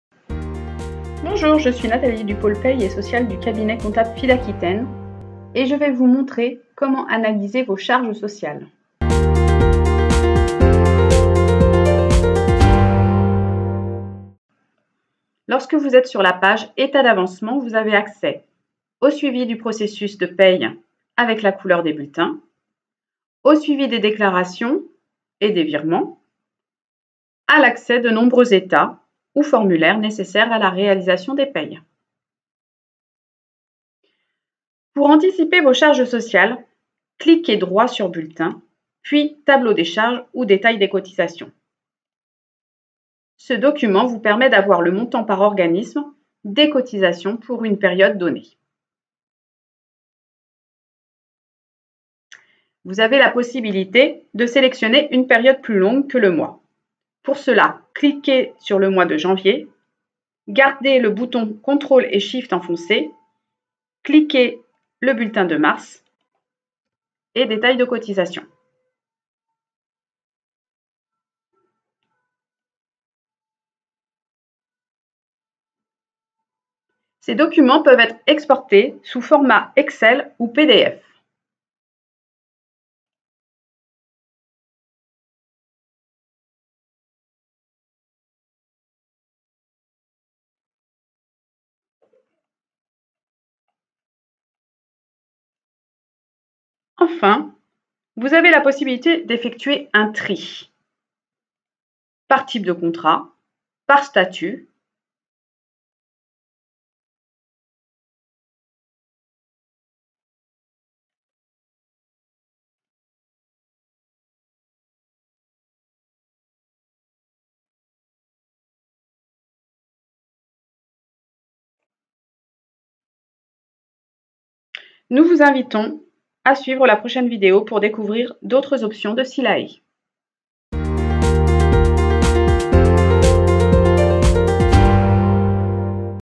SILAE - Comment analyser les charges sociales ?
Fidaquitaine met à disposition de ses clients le portail SILAEXPERT. Ce logiciel collaboratif sert à la gestion de la paie et des ressources humaines.

Dans cette vidéo, nous vous expliquons comment accéder à la réalisation des paies.